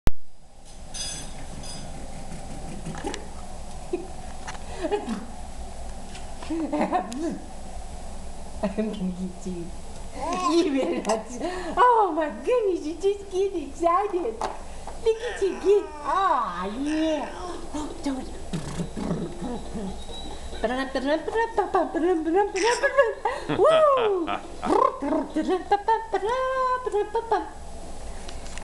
rida horsey
Jacob playing on his horsey with Gram